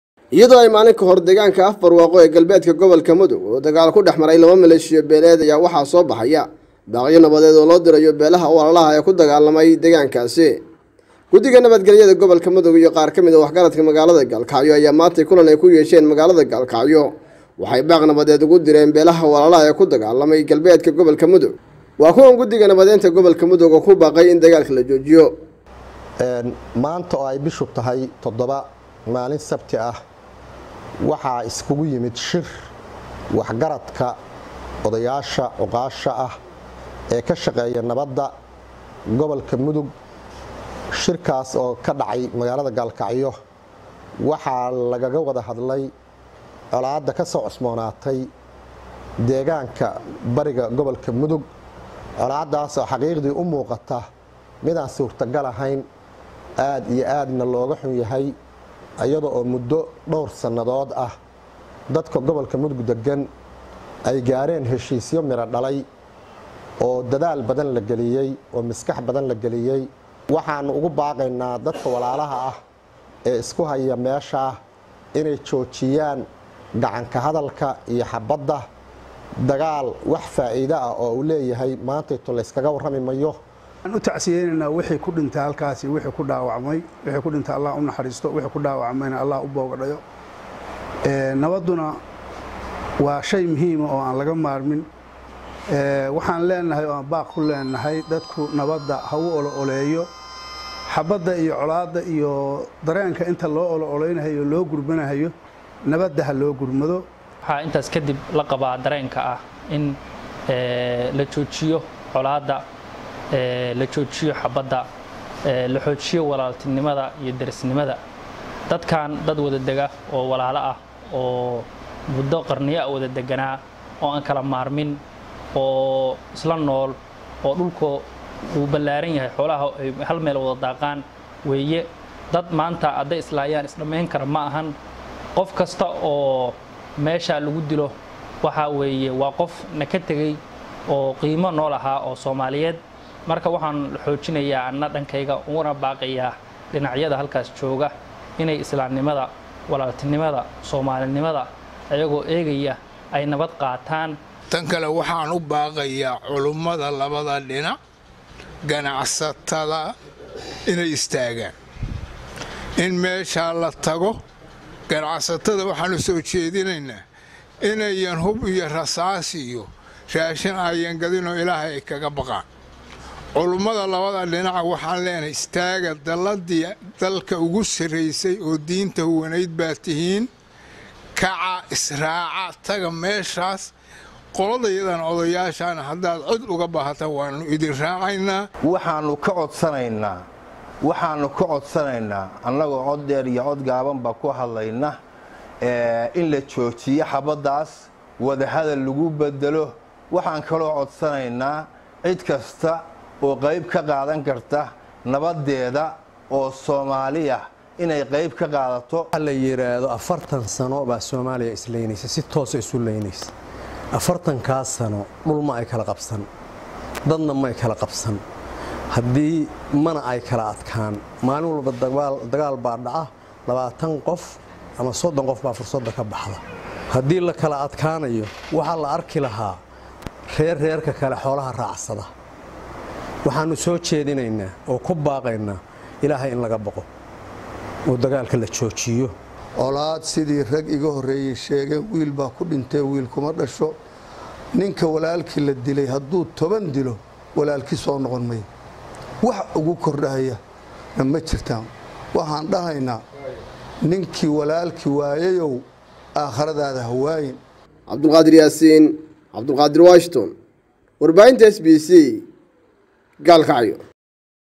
Gudiga nabadeynta Gobolka Mudug oo baaq nabadeed soo saaray
GUDIGA NABADAYNTA GOBOLKA MUDUG OO BAAQ NABADEED U DIRAY BEELO KU DAGAALAMAYA MUDUG.